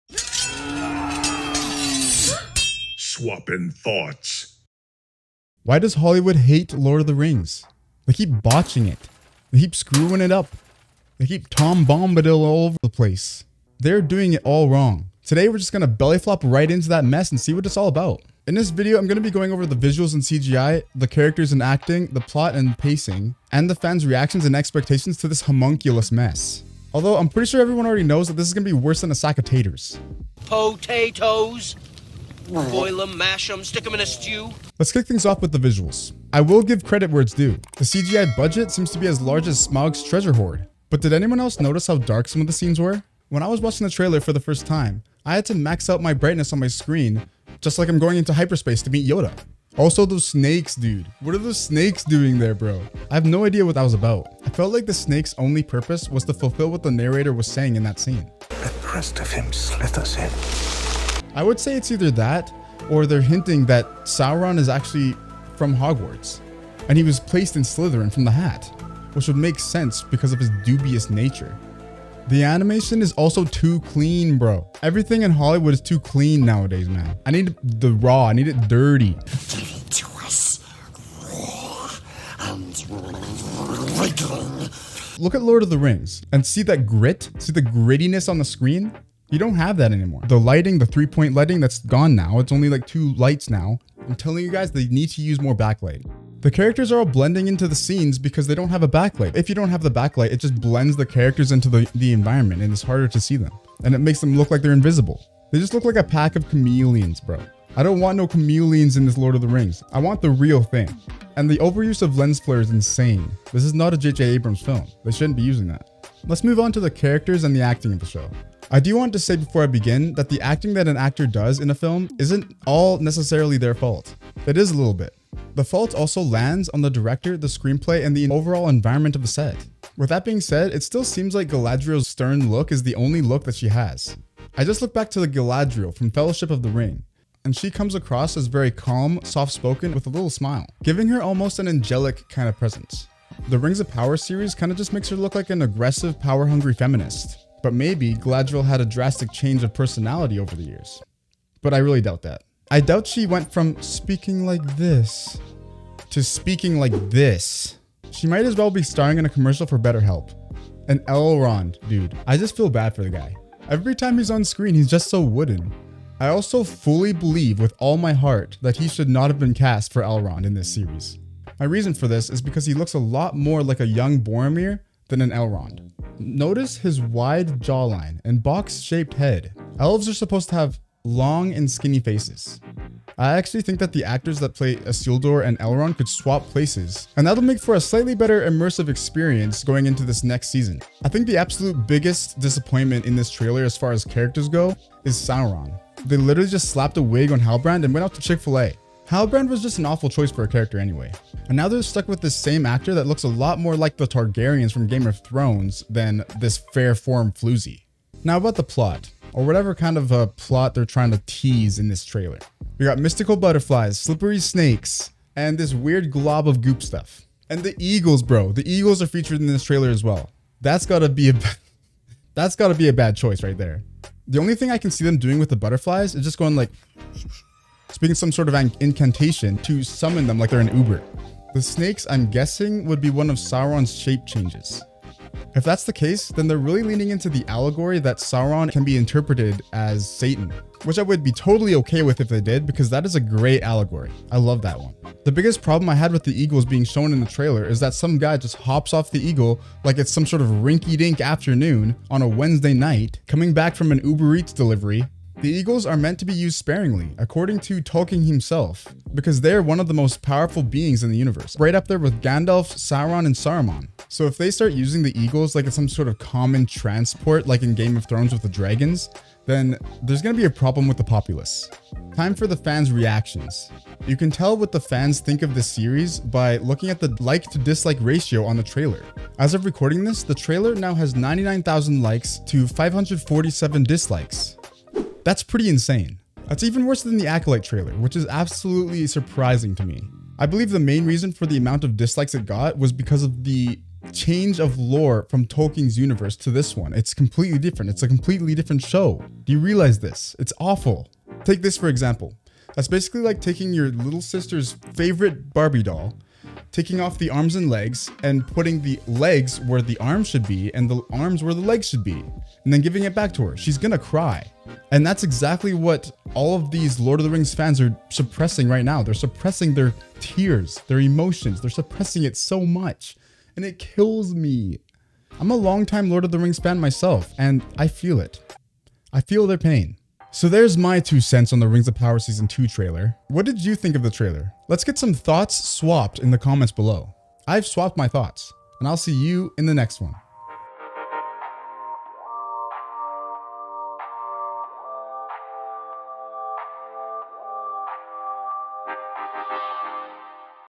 Rings of Power Season 2... Another Disappointment (Here's Why)
Here we go again... another disappoint from Hollywood. In this video, we summon all our big brain energy to critique the trailer for The Rings of Power season 2.

↦ Lord of The Rings Playing Cards (Amazon)

Timestamps:
0:00 - Intro
0:34 - Visuals & CGI
1:56 - Characters & Acting
3:28 - Plot & Pacing
4:32 - Fan Reactions
5:30 - Conclusion